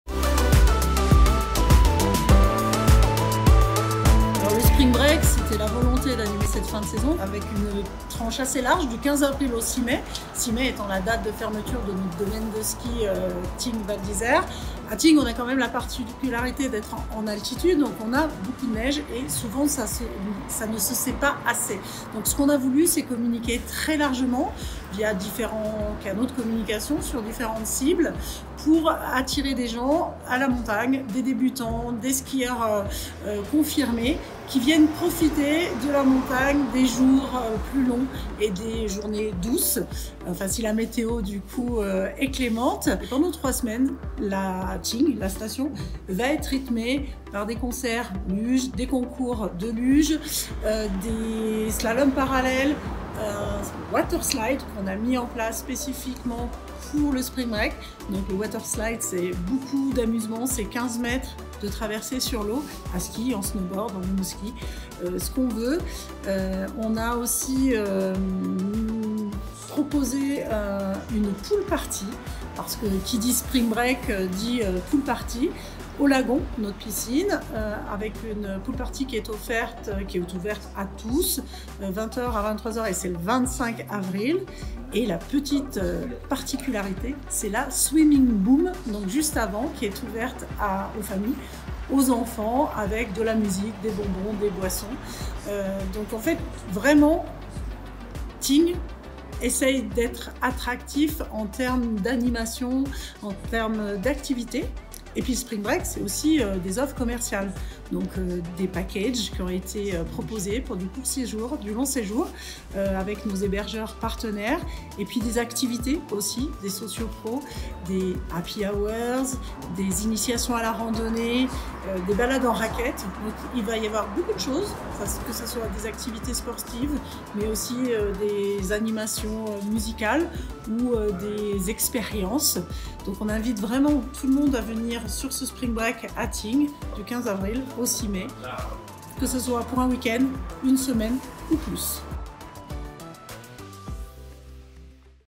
Tignes : Première édition du Spring Break
ÉVÈNEMENT - Première édition du Spring Break de Tignes ⛷️

Pour terminer la saison, la station de Tignes organise, du 15 avril au 6 mai 2023, la première édition du Spring Break. Pendant 3 semaines, de nombreuses activités sont proposées : concerts, courses de luge, waterslide, courses d'obstacles, pool party géante… Pour connaitre le programme complet, rendez-vous

Images : Tignes